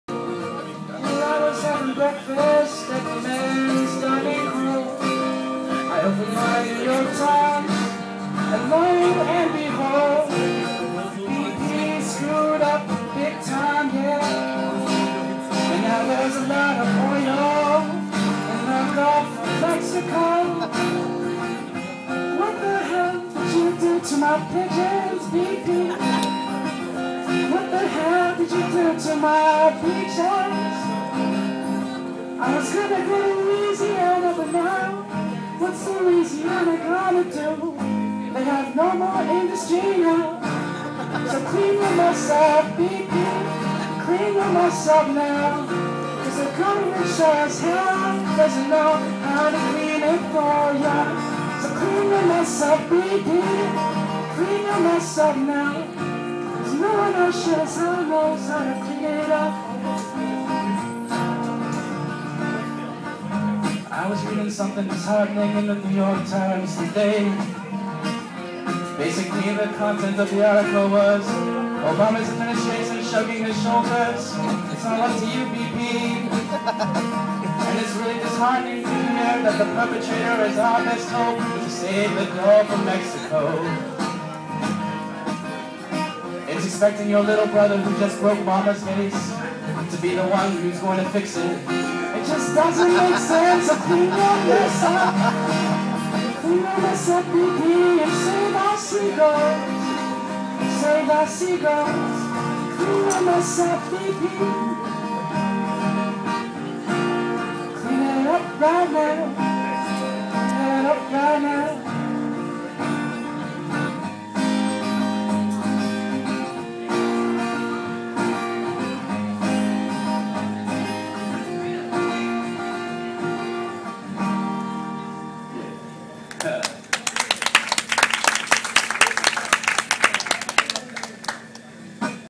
BP Oil Spill - Andrew Plan - Musical Improvisation @ CoHo Cafe Stanford
Live musical improvisation about the oil spill caused by BP and their negligence in the Gulf of Mexico. Recorded live on Tuesday May 25th 2010 at the CoHo Cafe Stanford.